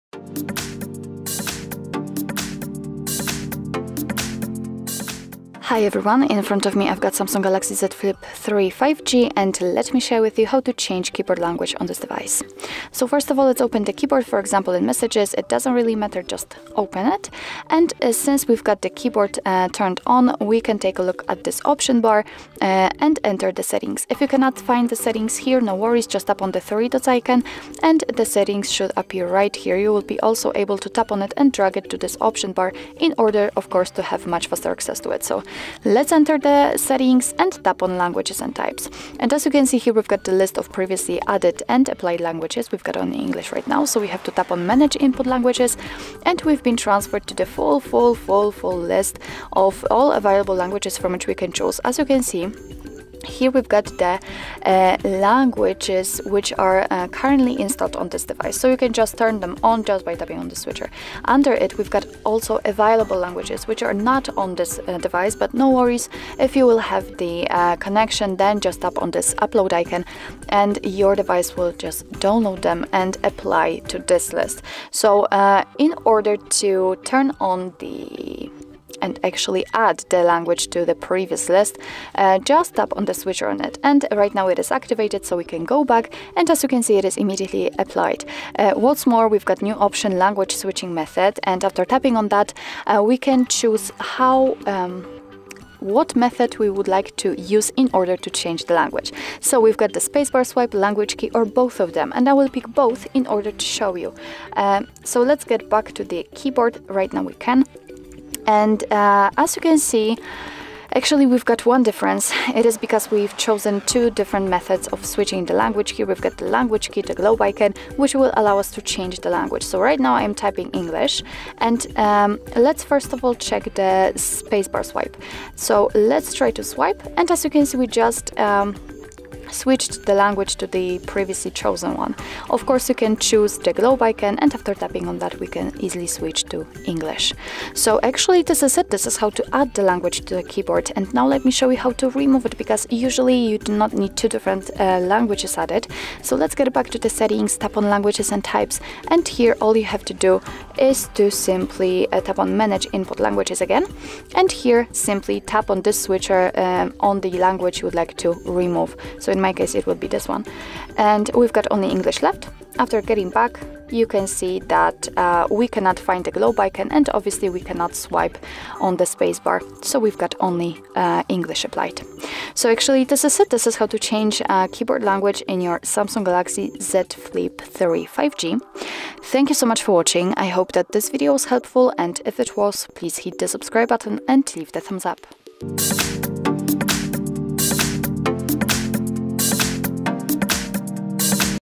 How to Change Keyboard Language in SAMSUNG Galaxy Z Flip3 5G - Keyboard Settings
Learn more info about SAMSUNG Galaxy Z Flip3:
Would you like to know how to change the keyboard language in SAMSUNG Galaxy Z Flip3 5G? Let's open this tutorial. Our expert will show you how to enter the keyboard settings and then how to open the list of languages that you can add to the keyboard. Choose a new keyboard language on SAMSUNG Galaxy Z Flip3 5G or if you want you can add more languages and switch freely between them.